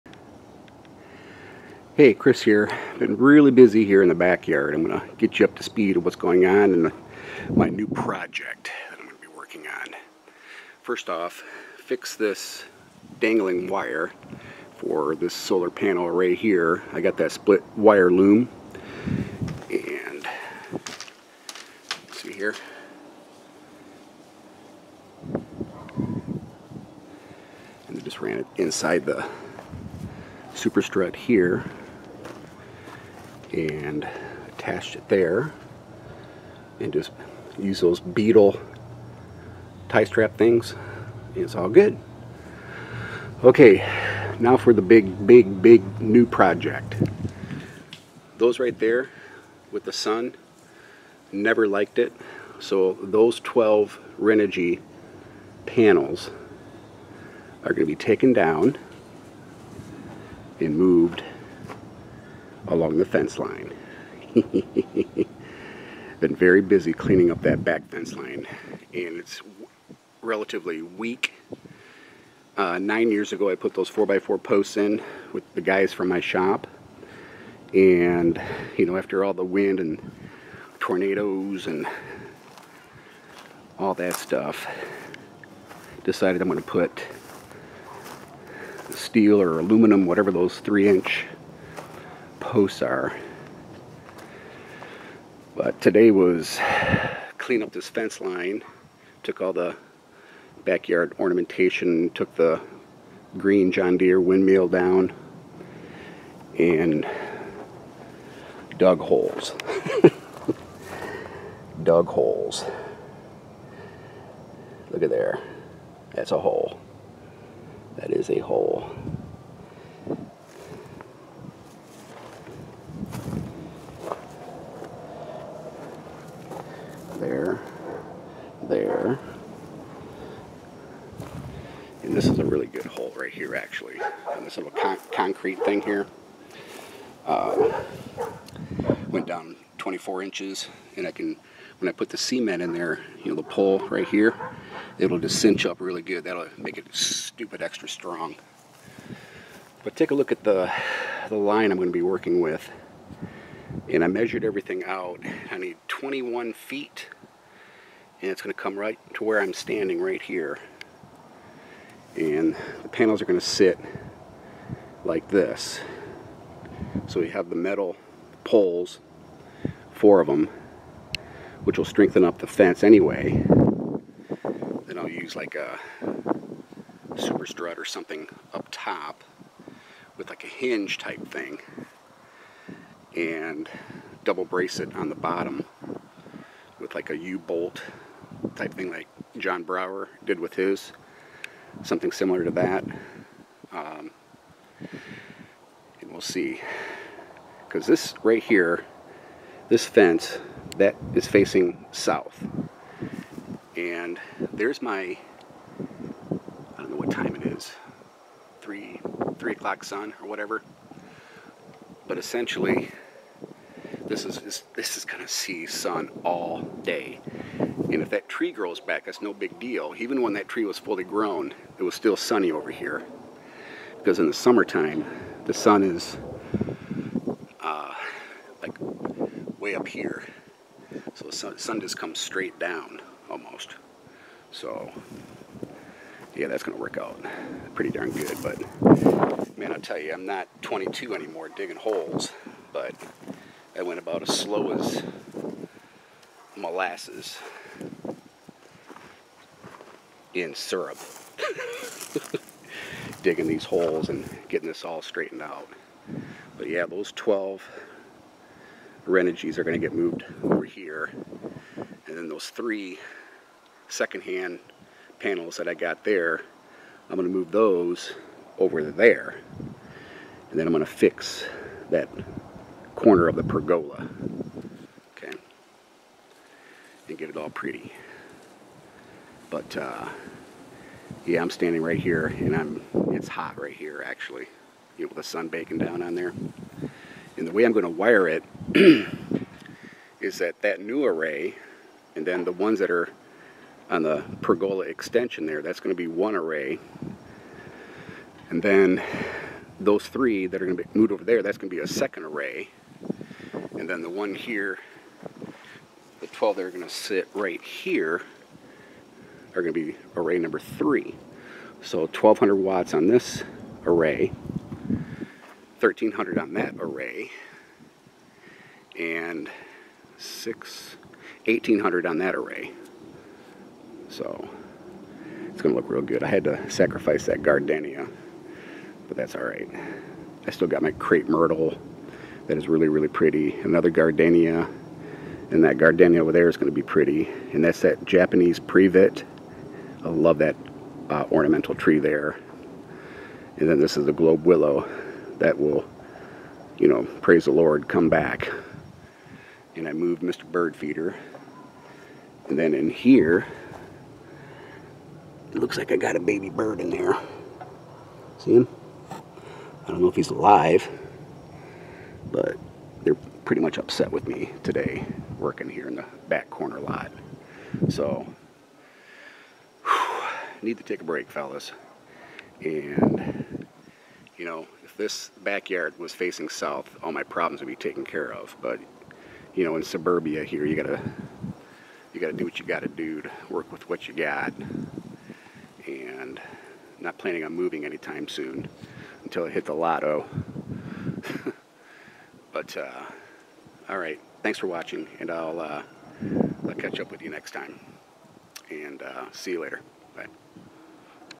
New Project - Moving 12 Renogy solar panels off of the pergola and onto the fence
Two things: make that fence stronger and mount solar panels on metal posts. The holes are dug. Hard packed clay and also moist clay and dirt. Making things better.